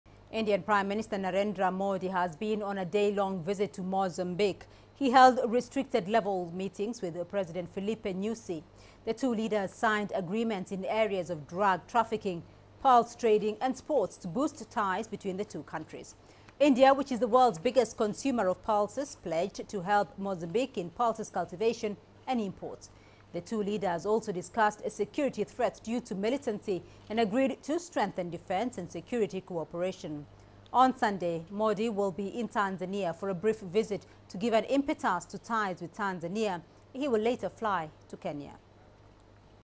India and Mozambique sign agreements
Indian Prime Minister Narendra Modi has been on a day-long visit to Mozambique. He held restricted-levels meeting with President Filipe Nyusi. The two leaders signed agreements in areas of drug trafficking, pulse trading and sports to boost ties between the two countries.